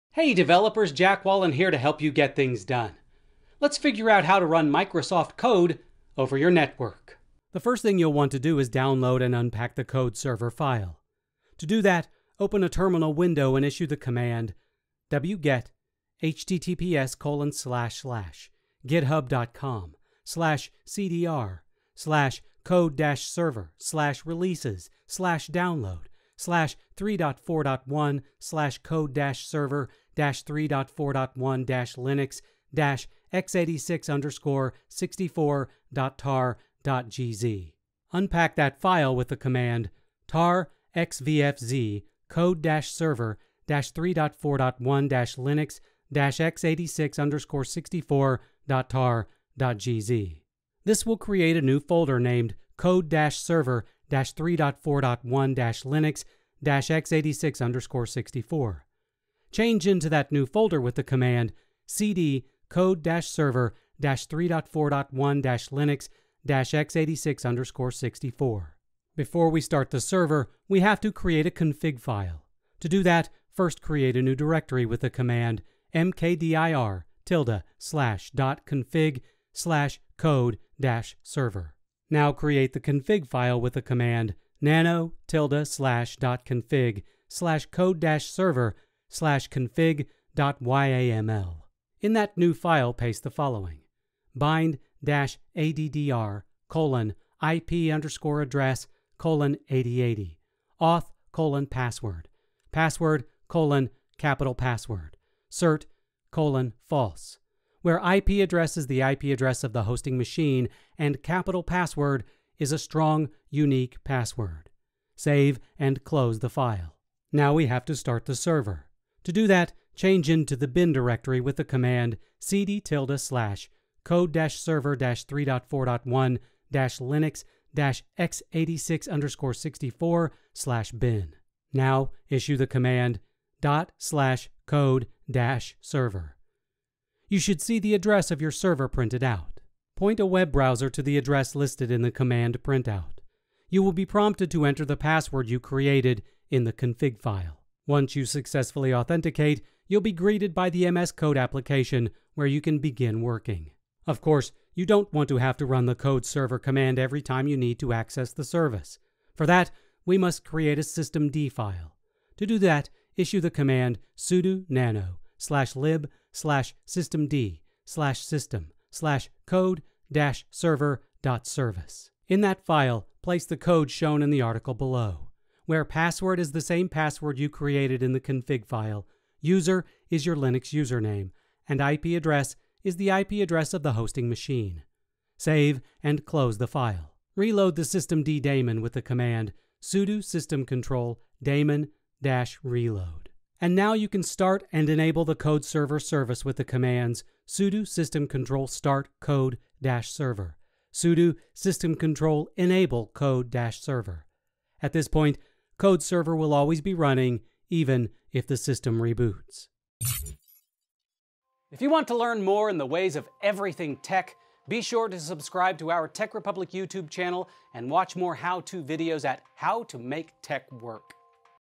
How to run an instance of Visual Studio Code over your network
With just a bit of cobbling together, you can have a networked instance of Visual Studio Code up on your LAN.

List of Commands:

tar xvzf code-server-3.4.1-linux-x86_64.tar.gz
cd code-server-3.4.1-linux-x86_64
mkdir -p ~/.config/code
nano ~/.config/code-server/config.yaml
bind-addr: IP_ADDRESS:8080
auth: password
password: PASSWORD
cert: false
cd ~/code-server-3.4.1-linux-x86_64/bin
./code-server
sudo nano /lib/systemd/system/code-server.service
[Unit]
Description=code-server
After=nginx.service

[Service]
Type=simple
Environment=PASSWORD=password
ExecStart=/home/USER/code-server1/code-server-3.4.1-linux-x86_64/bin/code-server --bind-addr IP_ADDRESS:8080 --user-data-$
--auth password
Restart=always

[Install]
WantedBy=multi-user.target
sudo systemctl daemon-reload
sudo systemctl start code-server
sudo systemctl enable code-server